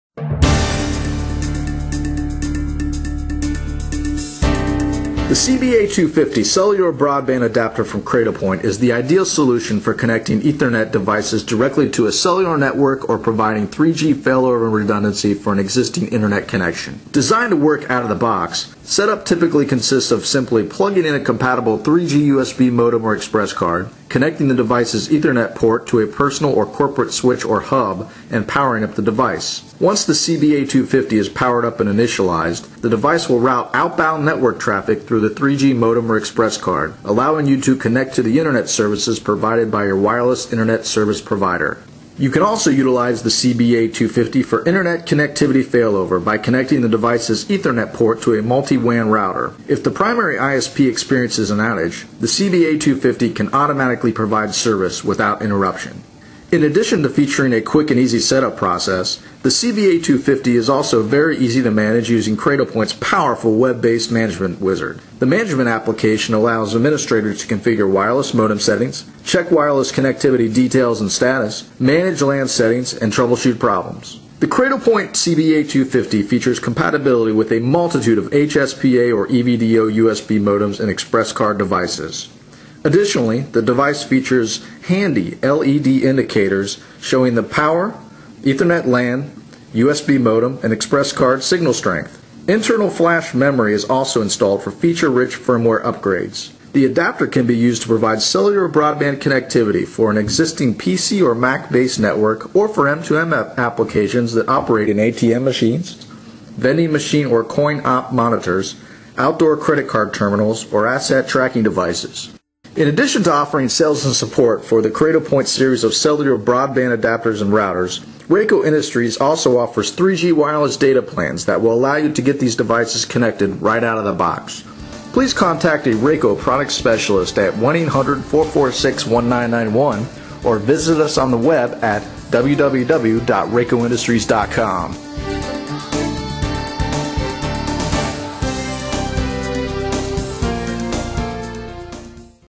Cradlepoint CBA250 Cellular Broadband Adapter and Router for 3G USB and ExpressCards
The CBA250 from Cradlepoint allows multiple ethernet devices to share a 3G connection (USB or Expresscard), can act as a wireless failover for existing home and business Internet connections and can connect existing ethernet devices to 3G wireless GSM or CDMA networks.  This is ideal for ATM's, M2M asset tracking and remote monitoring applications.